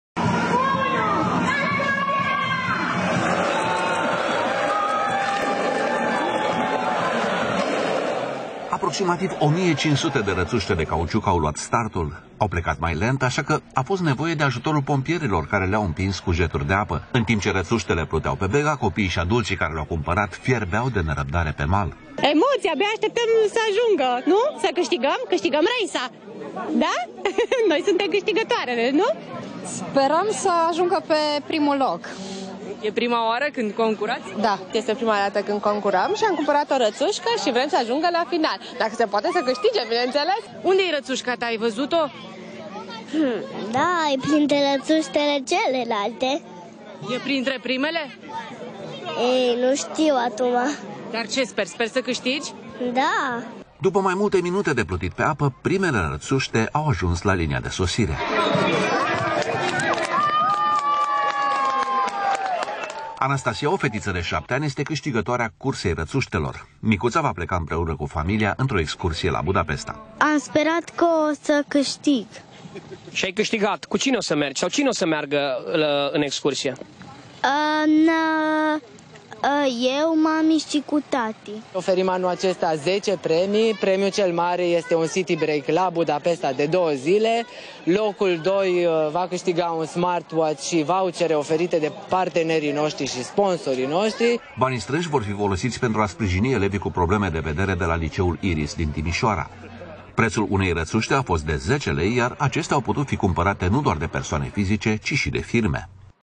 Cursa rățuștelor de cauciuc, reluată pe Bega.
Aproximativ 1500 de jucării au luat startul